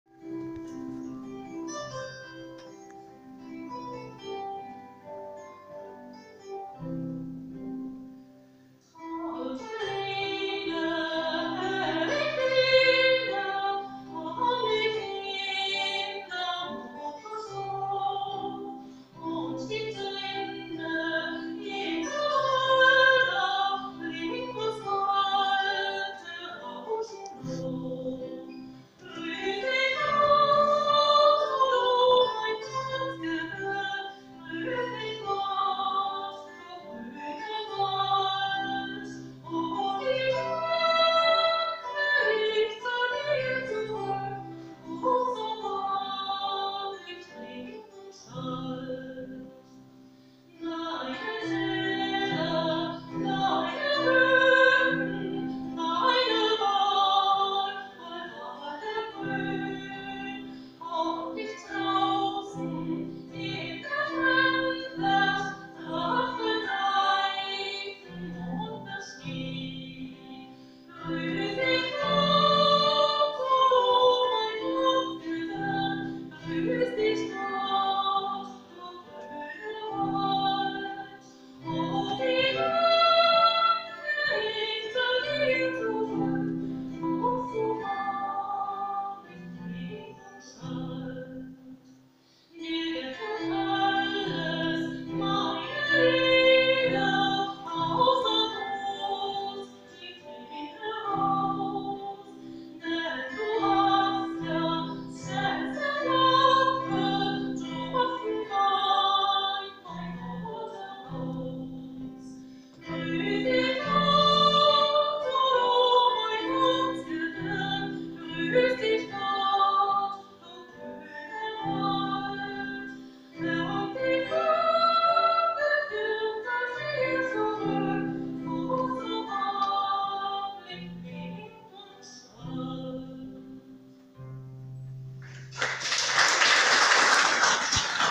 VID 20200829 171238 Duo Zitherklang
Sommermusiken in der Seiffener Kirche, hier das Duo Zitherklang aus Freiberg, Gesang: Birgit Weber. Entschuldigung für meinen schlechten Handyton!